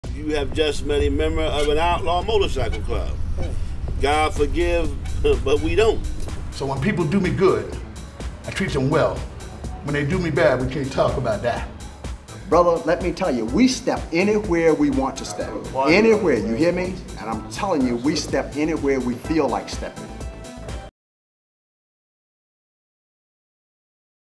Wheels of Soul
Wheels of Soul is a racially mixed 1% outlaw motorcycle club